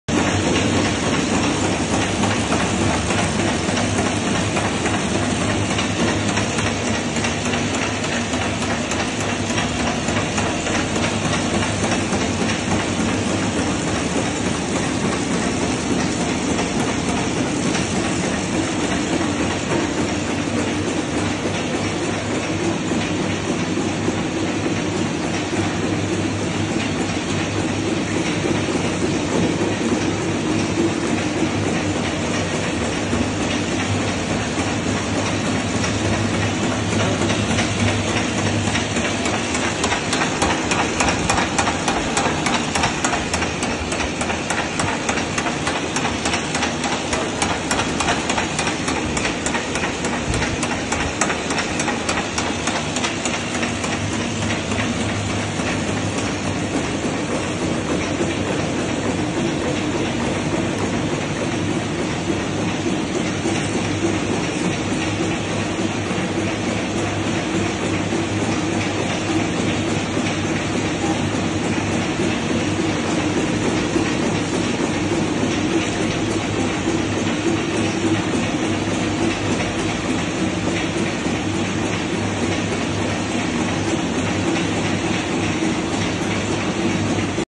oyuncu kartı paketleme makinası otomatik ( football varda packaging)
Oyuncu kartları , kartvizit ve benzeri ürünler için paketleme makinaları.otomatik besleme sistemli dakikada 200 paket kapasite

Football cards and business card packaging machine with automatic feeding system.